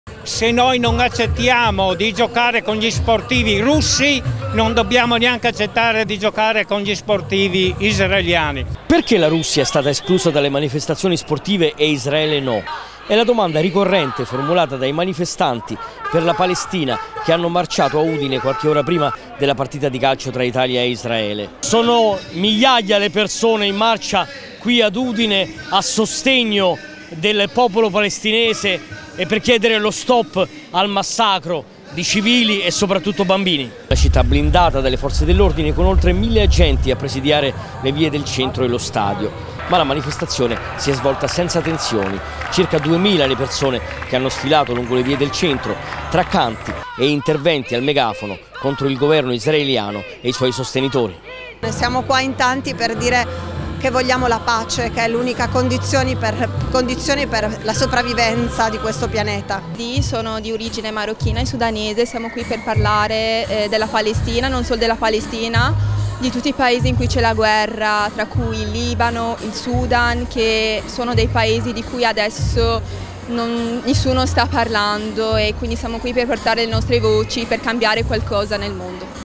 "Perché la Russia è stata esclusa dalle competizioni sportive e Israele no?" Il quesito dei pro Pal
Sono stati più di duemila i manifestanti pro Palestina che hanno sfilato a Udine contro la partita di Nations League Italia-Israele disputata al Bluenergy Stadium del capoluogo friulano. Iniziata verso le 17 e terminata intorno alle 20 in piazza XX Settembre, si è svolta senza incidenti.
Si sono levate critiche per il patrocinio dato da Regione Friuli e Comune di Udine al match. Con una domanda ricorrente: "perché la Russia è stata esclusa dalle manifestazioni sportive e Israele no?"